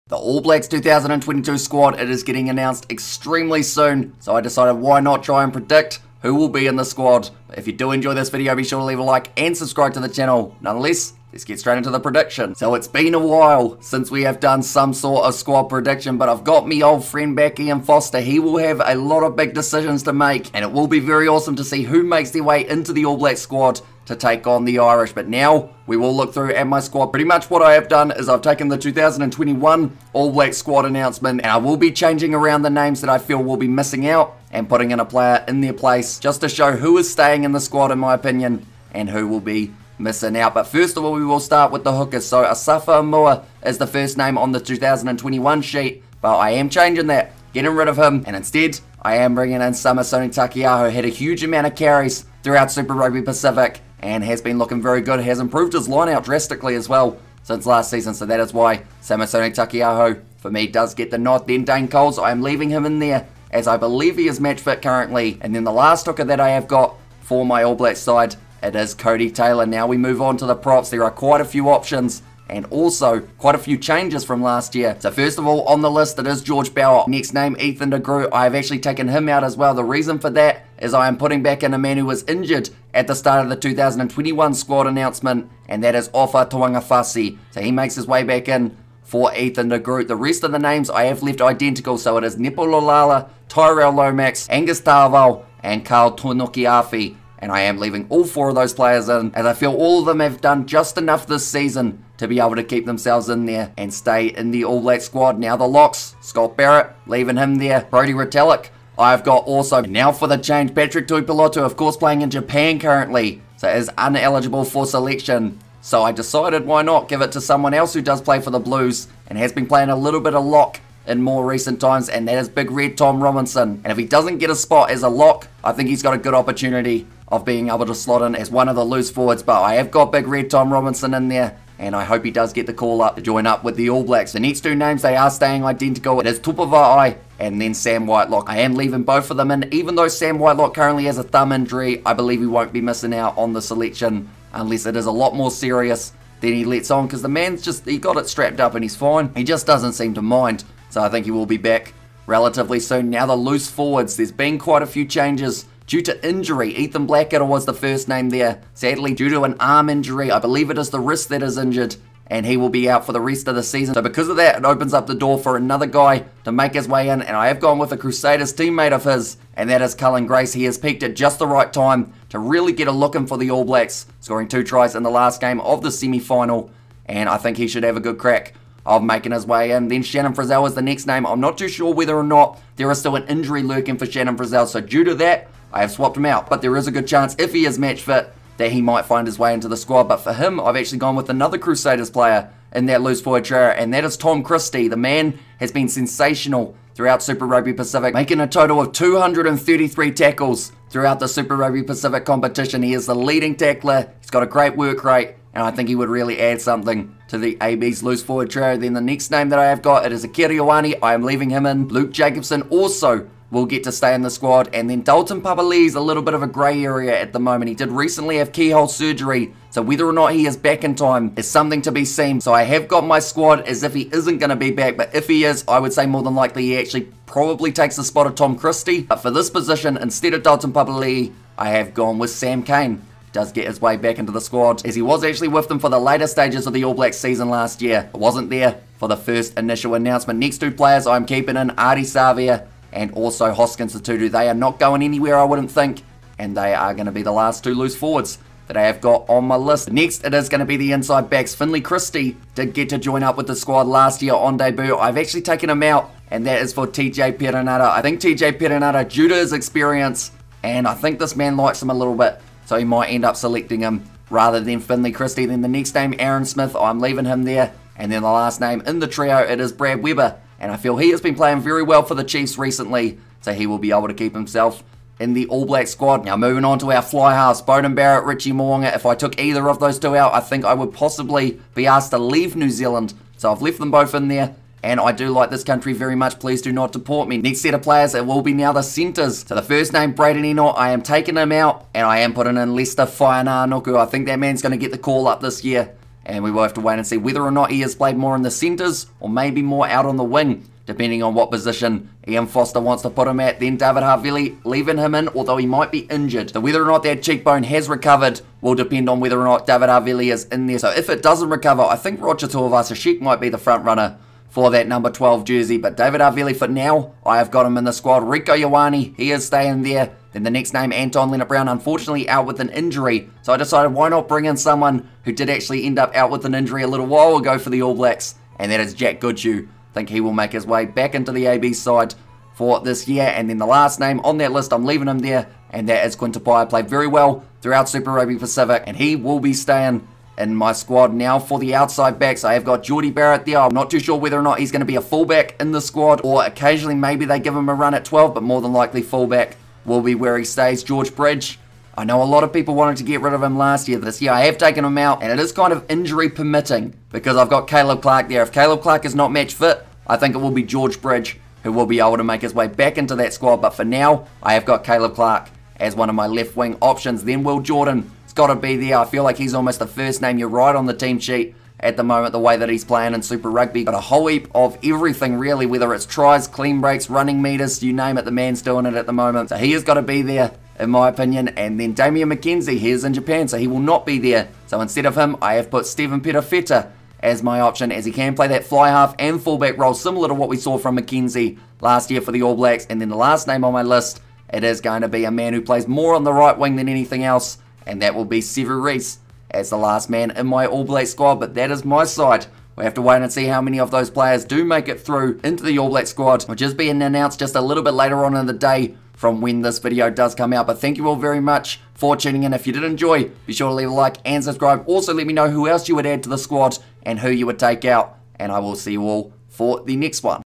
Predicting the ALL BLACKS 2022 Squad to Play IRELAND!
In this video I try to predict the All Blacks 2022 Squad that will facing Ireland in July. The All Blacks 2022 Squad is being announced later today so it seems like the perfect time to have a guess at the type of New Zealand Squad we could see for the upcoming New Zealand vs Ireland 2022 tour.

Who would you have in the All Blacks 2022 Squad?